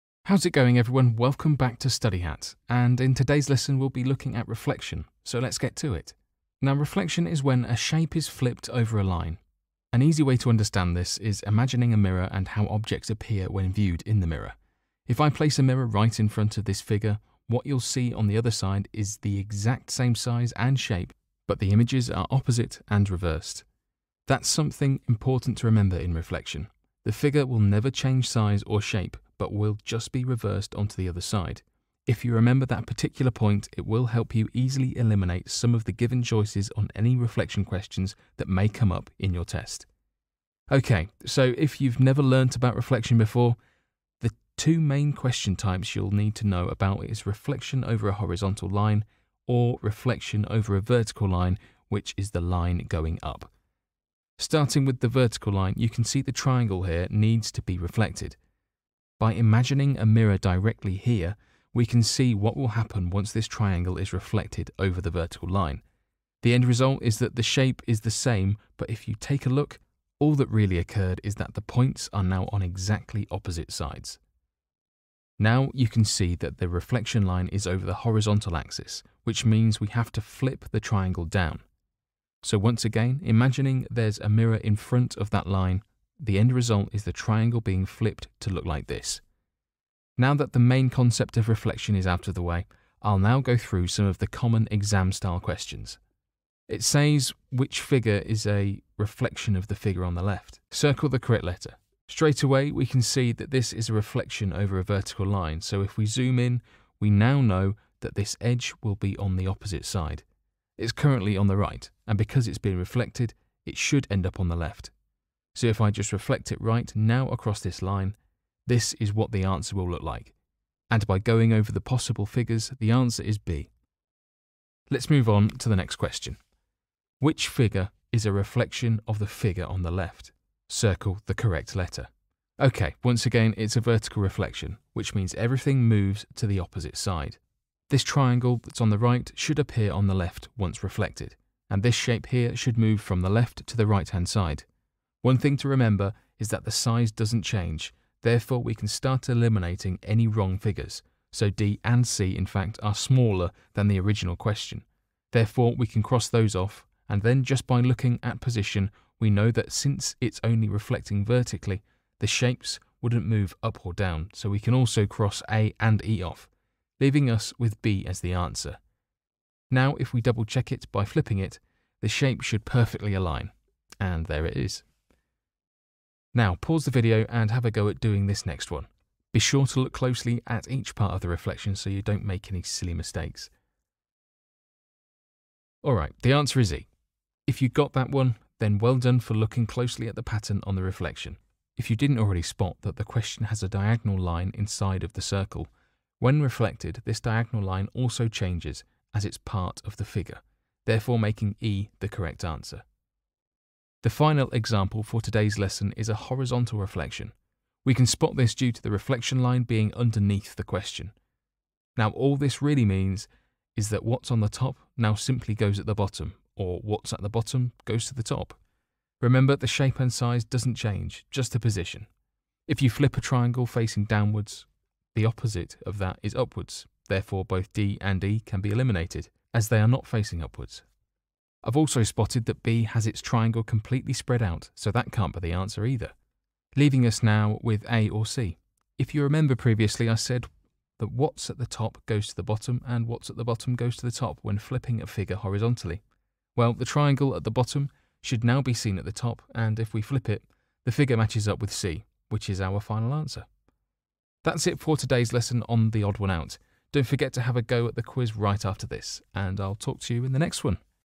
Eleven Plus Non Verbal Reasoning - Reflection
In today's video we'll be going through Reflection which is a topic of Non Verbal Reasoning that has frequently come up during the 11+ exam.